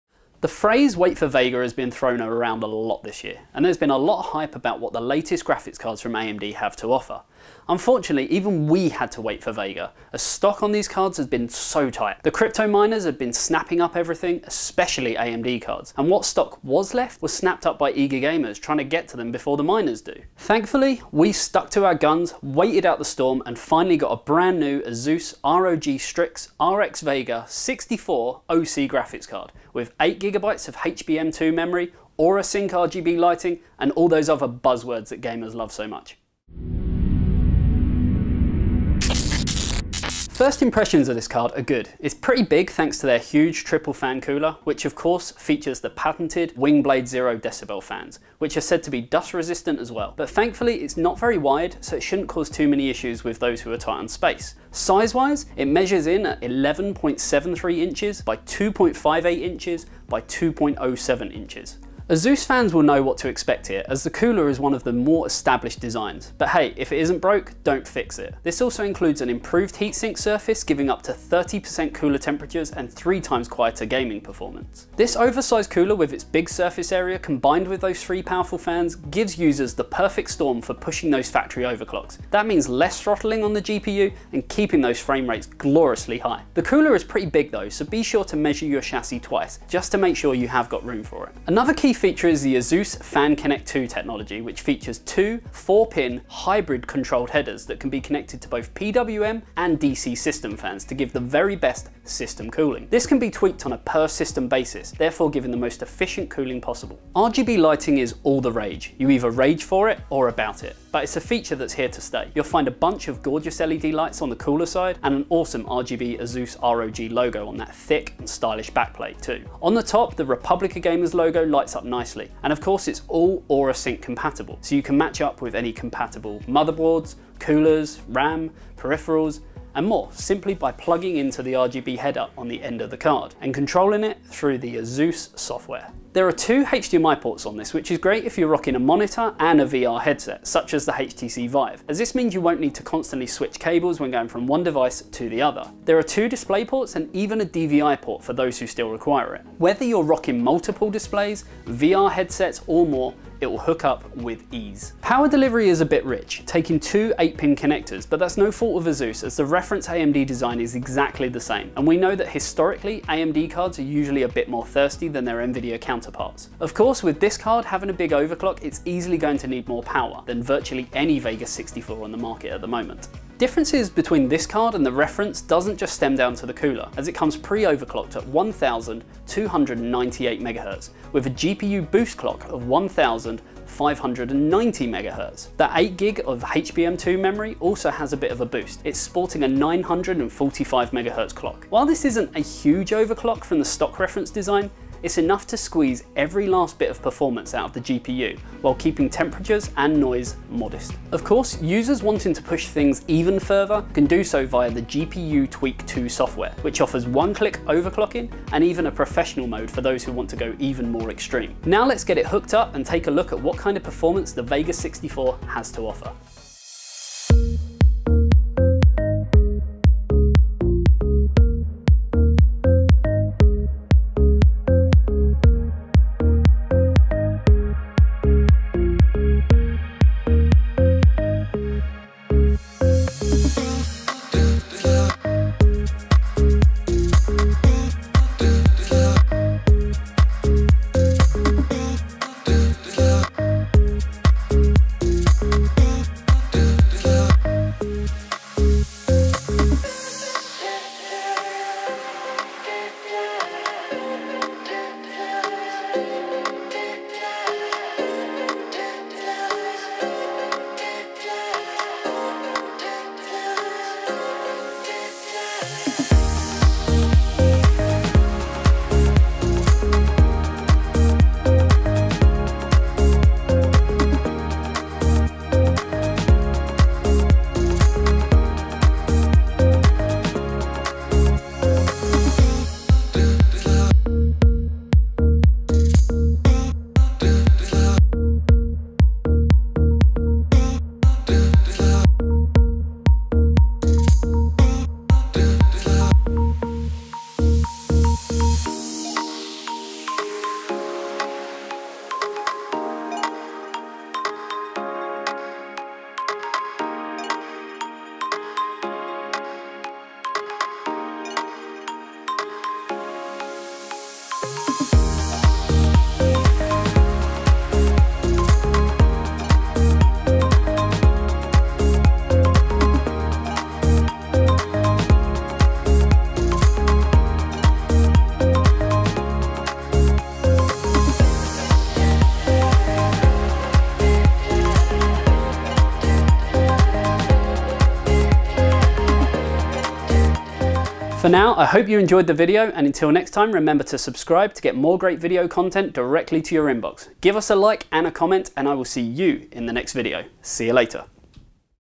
Asus STRIX Radeon RX Vega 64 Review - FINALLY A Custom Card!
With AMD releasing their latest VEGA graphics cards not to long ago, consumers were eagerly awaiting for custom cards and today is that day. We're taking a look at the ASUS STRIX Radeon RX VEGA 64 graphics card sporting a factory overclock and much more with thanks to the patented features you'd expect from an ASUS product.

►UK Links◄

►US Links◄

►Our Kit◄